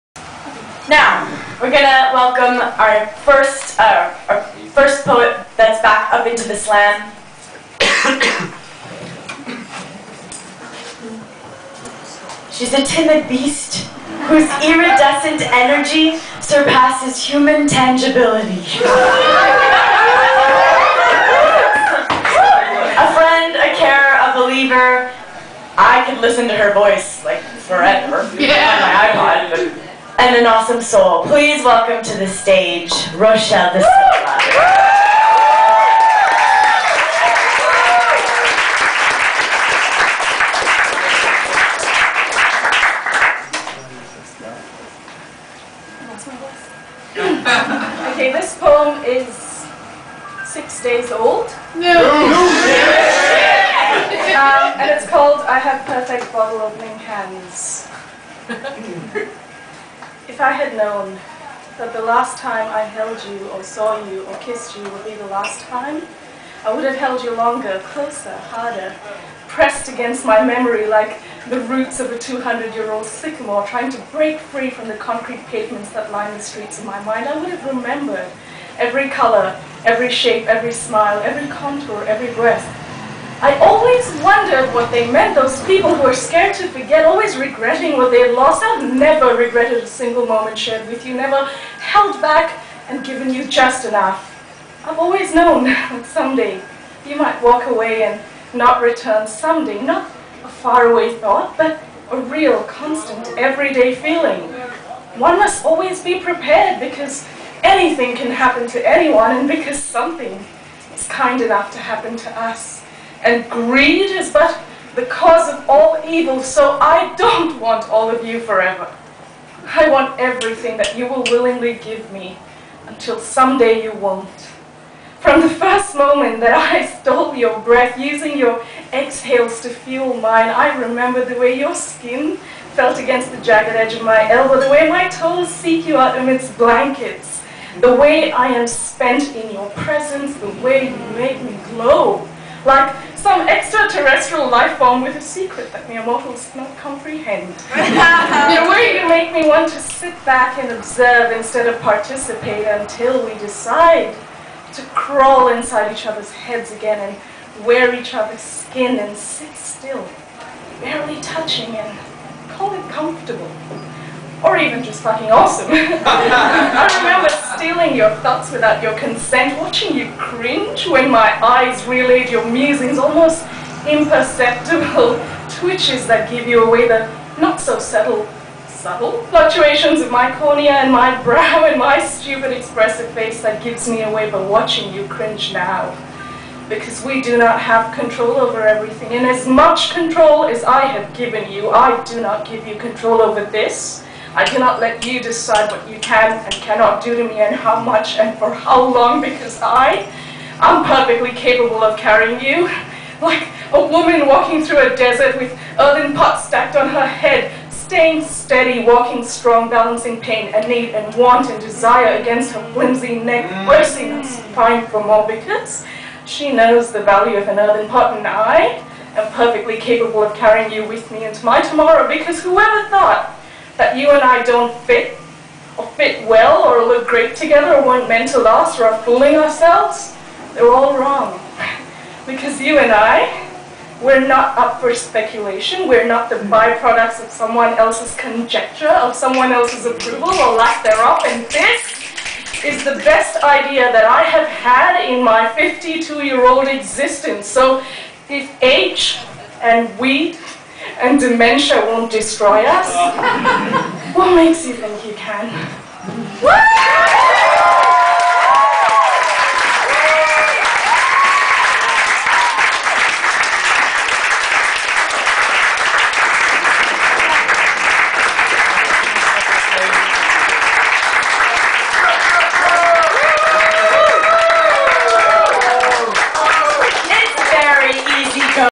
Rochelle D'Silva - I have perfect bottle opening hands
This is the piece that won me first place at my first ever poetry slam aka SLAMALAMADINGDONG! Poetry Slam. Check out my Facebook artist page for more info about gigs and more poetry